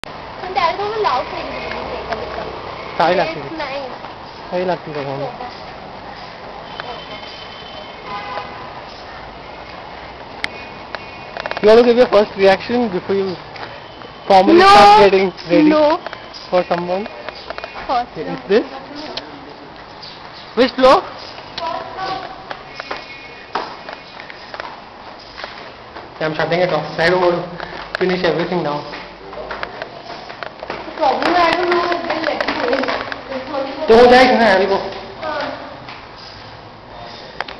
Runaway bride minutes before a tense make-up session
Following Vrinda to her parlour on the day of her engagement - Scene 3, Act 3